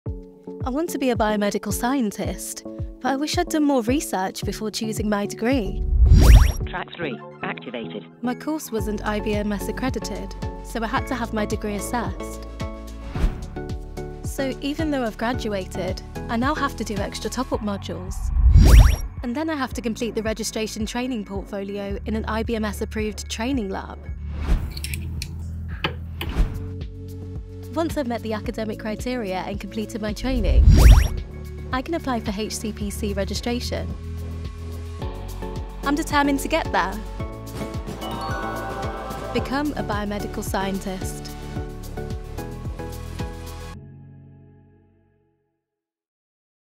Become a biomedical scientist - Track 3
Do you have a degree that is not IBMS Accredited but want to become a biomedical scientist in the UK?

This video highlights the IBMS route to HCPC registration as a biomedical scientist via a non-accredited degree: where you need to have an IBMS degree assessment, potentially do IBMS Accredited top up modules and also find a trainee position in an IBMS approved training laboratory where you can complete an IBMS Registration Training Portfolio.

Find out more by visiting: becomeabiomedicalscientist.com